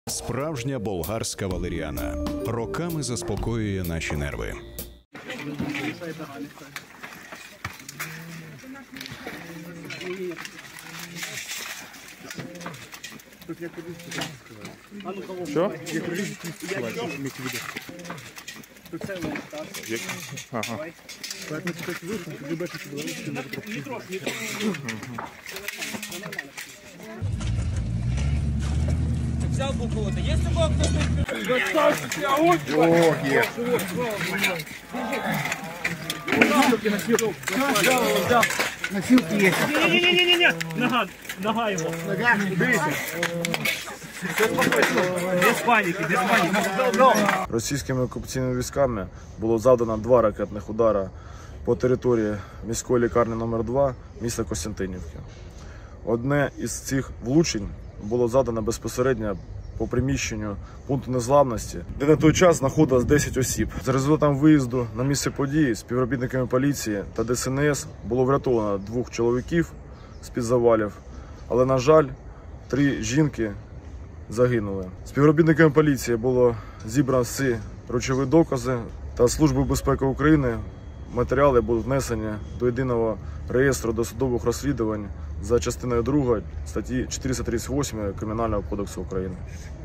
КОСТЯНТИНІВКА під ударом С-300 — перші КАДРИ ПОЛІЦІЇ
Російські війська вночі атакували Костянтинівку двома ракетами С-300. Ворог поцілив у "пункт незламності", де на той час перебували 10 мешканців Бахмутського району.

Відео: Поліція Донецької області